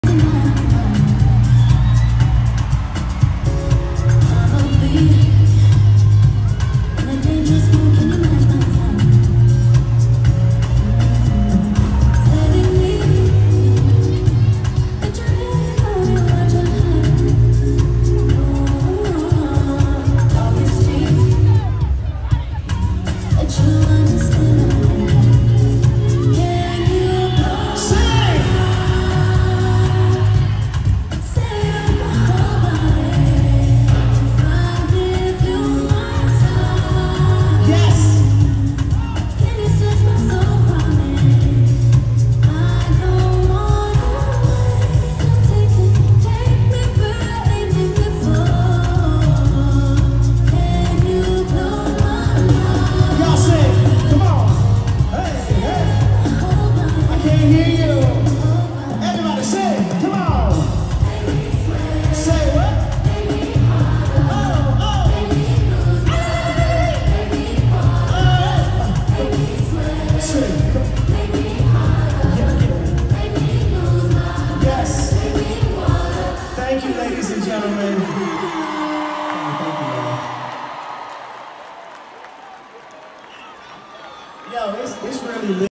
Usher showed major love to Tyla at the last Brooklyn stop of his USHER: Past Present Future tour
Should they drop a song together?